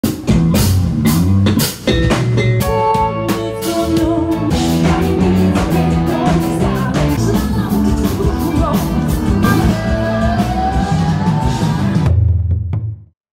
Tribute to Kora
Fragmenty koncertu dla wspaniałej publiczności w Chełmsku Śląskim (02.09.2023) maanam kora tribute tributeconcert tributesongs koncert jarmark muzykanażywo live mnich @kora @tributetokora2024 @TributetoKora